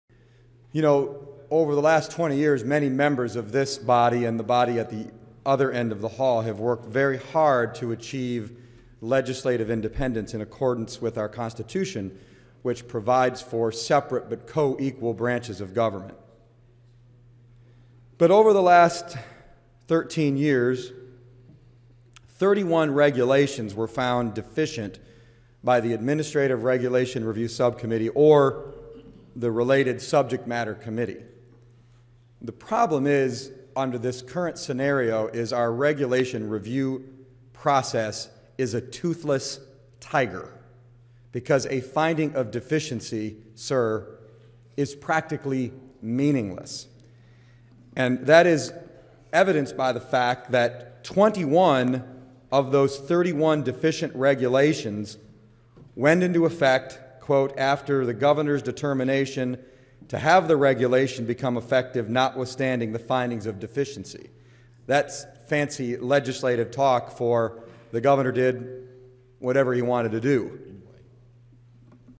Sen. Damon Thayer on Legislative Oversight
Senate Bill 1 proposes a constitutional amendment that would allow voters to decide whether legislators should have the power to void regulations issued by the executive branch. Majority Leader Damon Thayer (R-Georgetown) characterized the Administrative Regulations Review Subcommittee, the panel that sometimes finds regulations deficient, as a toothless tiger. He spoke on the Senate floor on February 6, 2014.